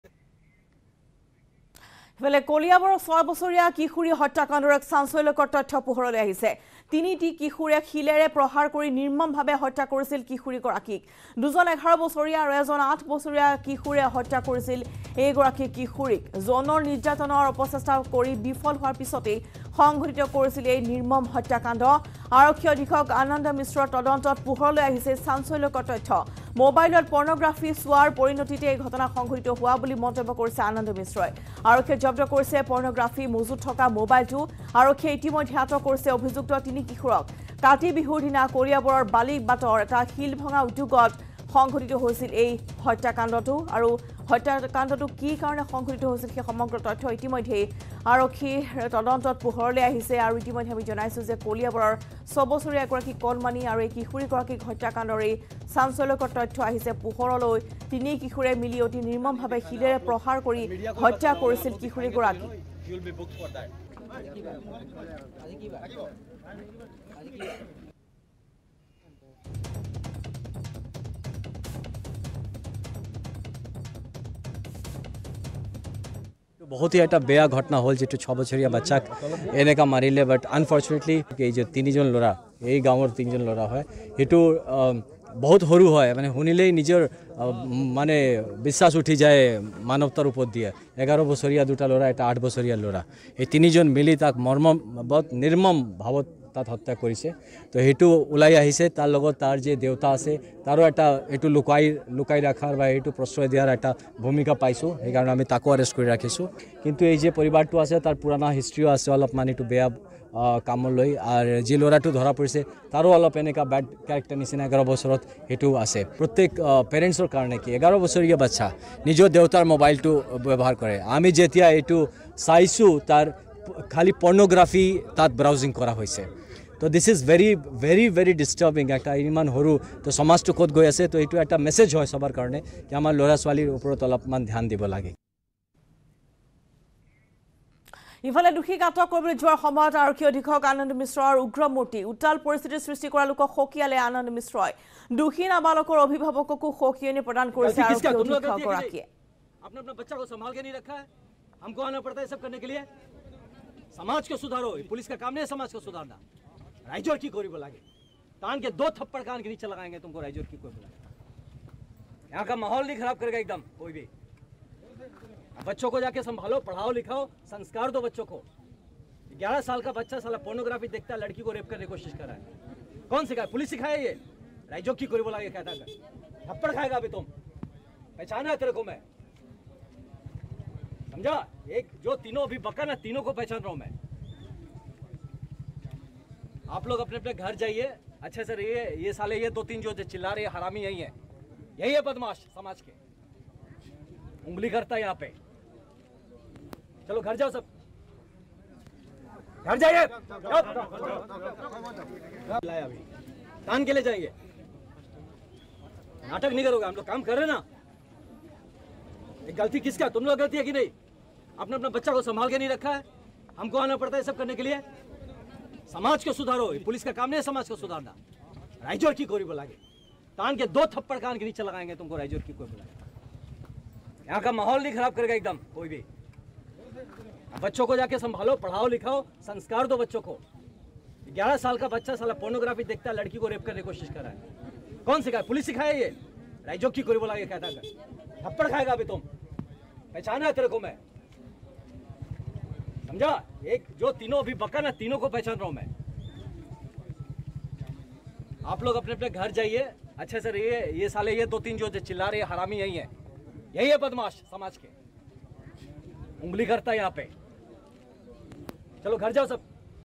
Kaliabor Breaking | তিনিটি কিশোৰে শিলেৰে প্ৰহাৰ কৰি নিৰ্মমভাৱে হত্যা কৰিছিল কিশোৰীক
21 October, 2021 : আৰক্ষীৰ জালত তিনি অভিযুক্ত কিশোৰ । লগতে চাওক ৰাজ্যৰ অন্যান্য খবৰ News18 Assam North Eastত